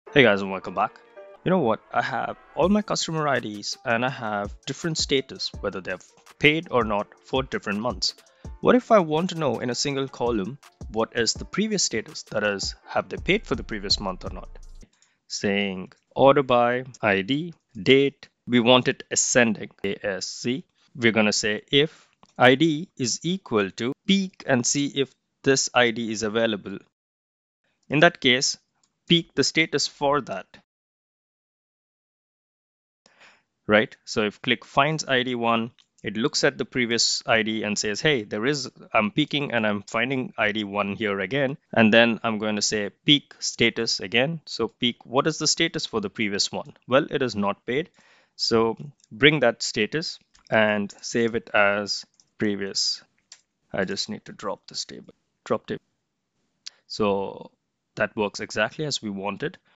Qliksense: Peek function trick. how to find previous month status.
🤖 Get the new AI & QlikSense : Integrating ChatGPT into Qliksense.

📚 Read on Kindle the simplest way to become ultra-productive: